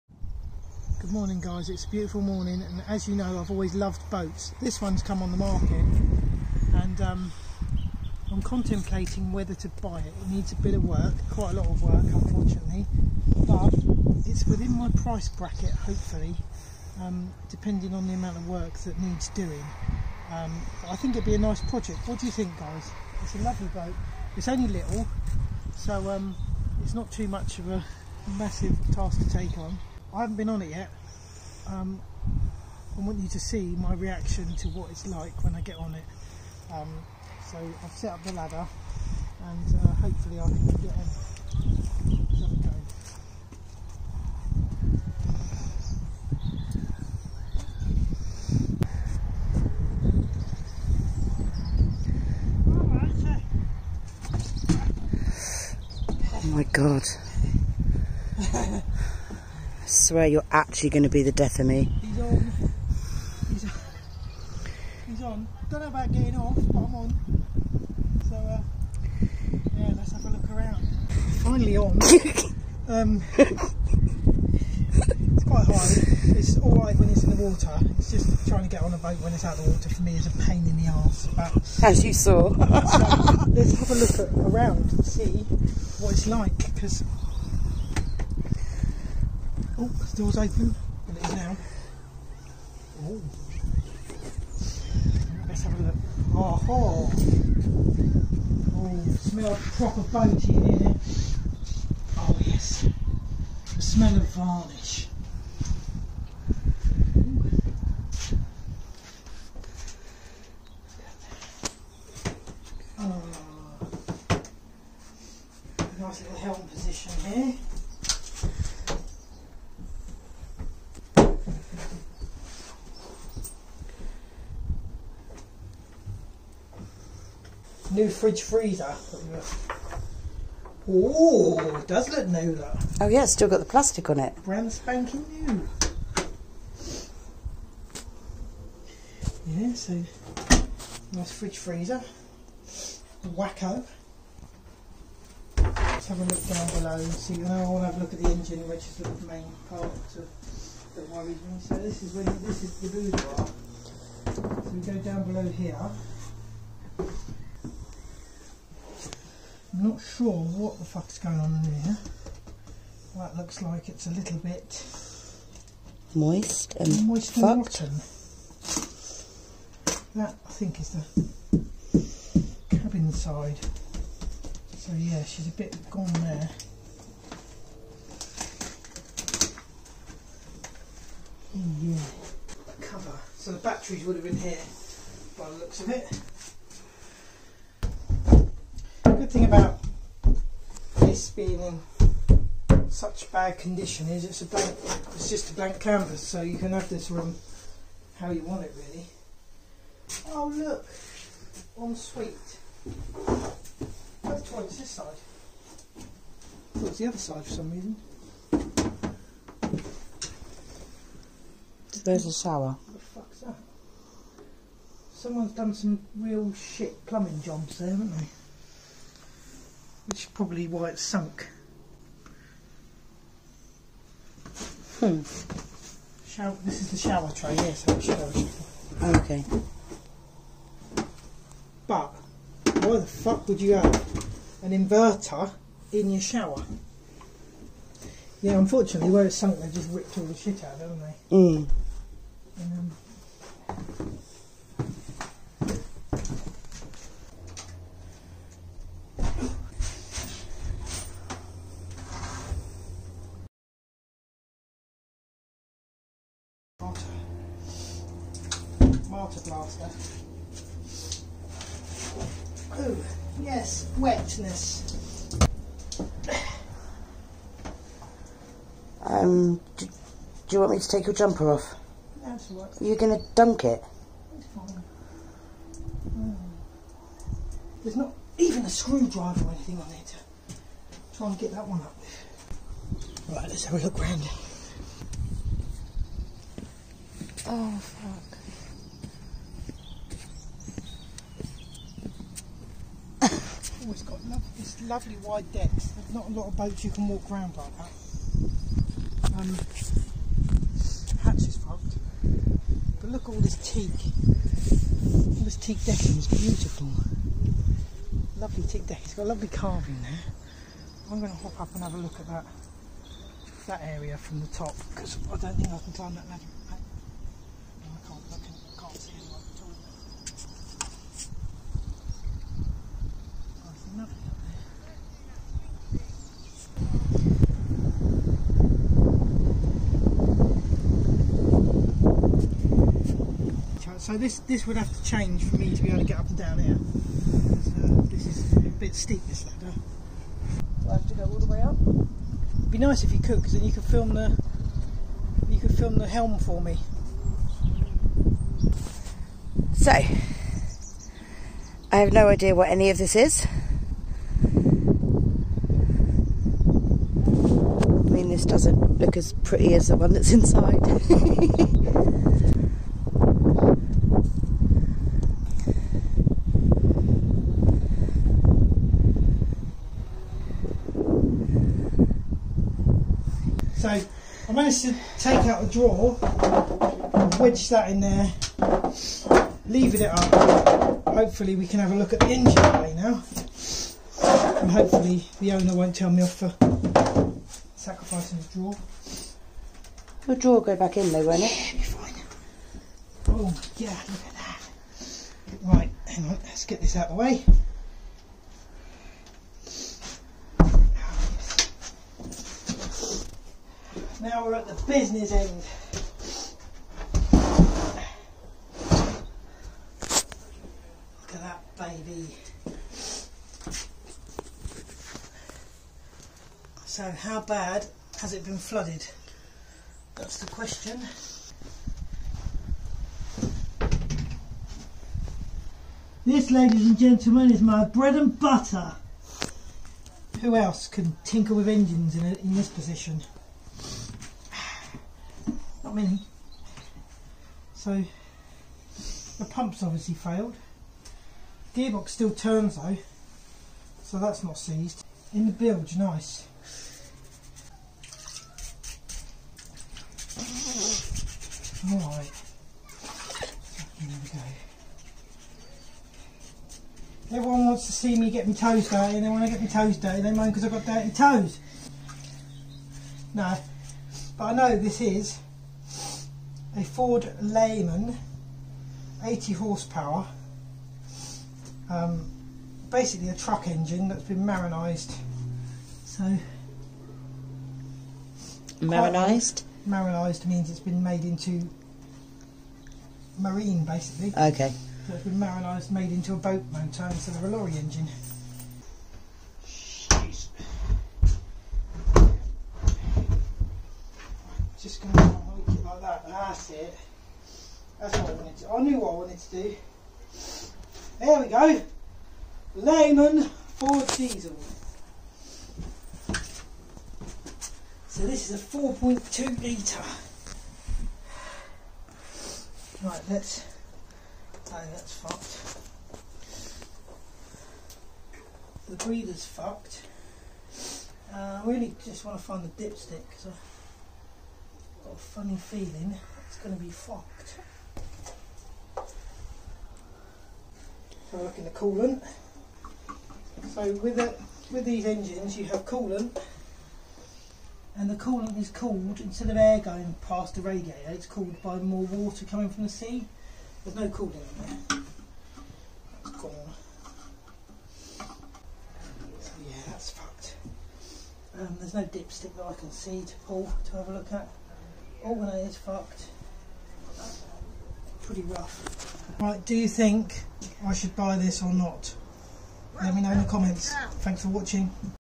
Thinking of buying this wrecked boat, which recently sank eurobanker trawler, what should I do
would you buy this. would you like to help me in this project, ? should I take the gamble? what should I offer ?